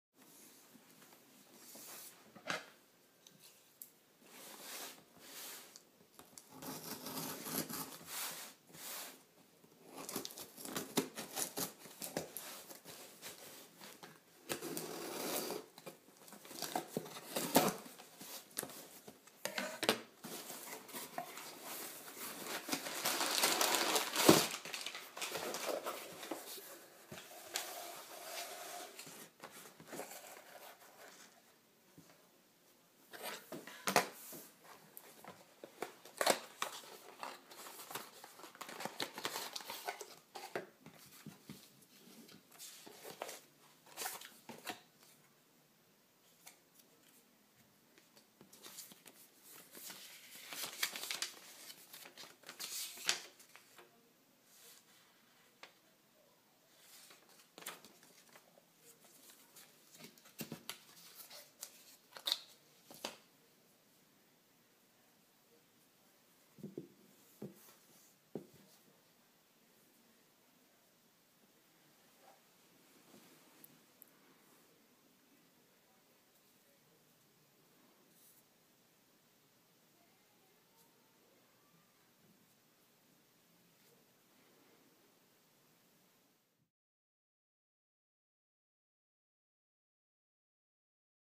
Cordies (new metal version) by Quirky
The new metal Cordies, by Quirky. They are a great little desktop accessory to help organize those annoying cords.